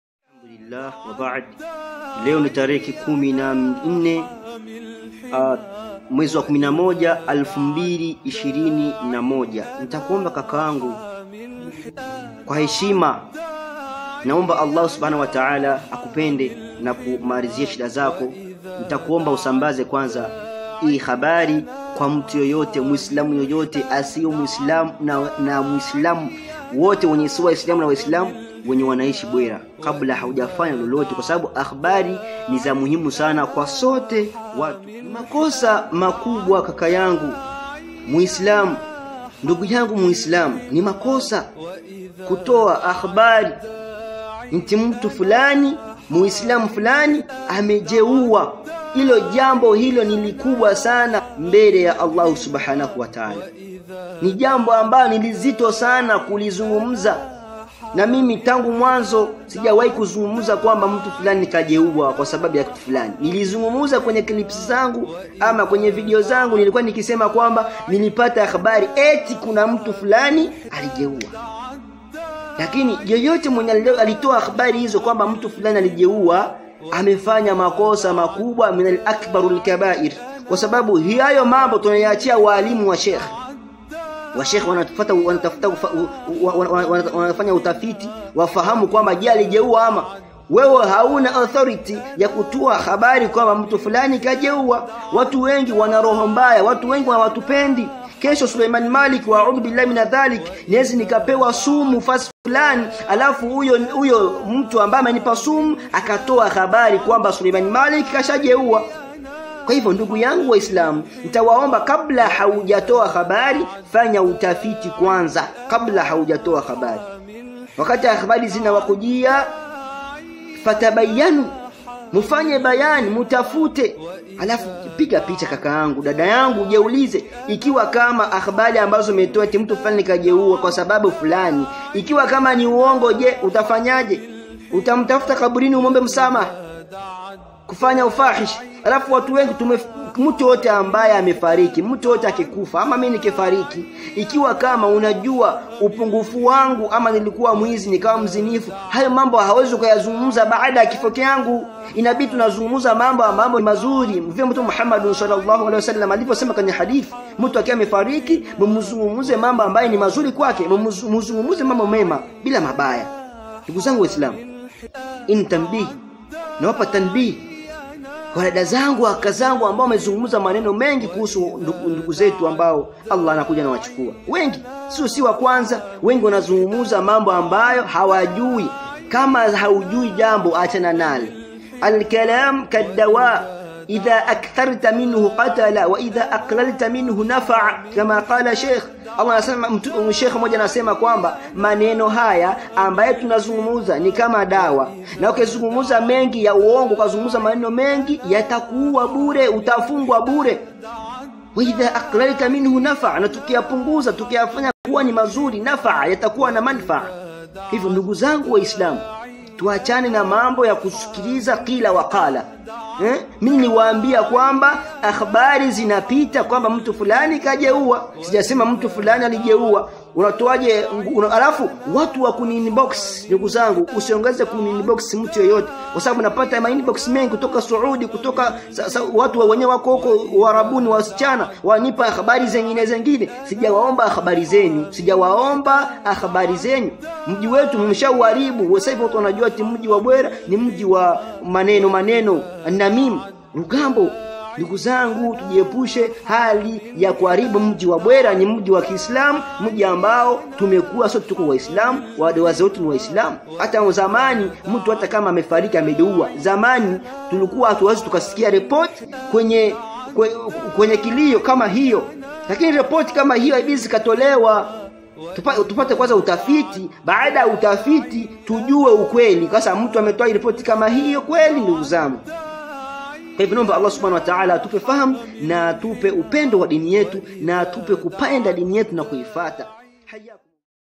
Fanya kwanza utafiti qabla ya qauli By Sheikh Sulaiman Maalik UG
bwera islam media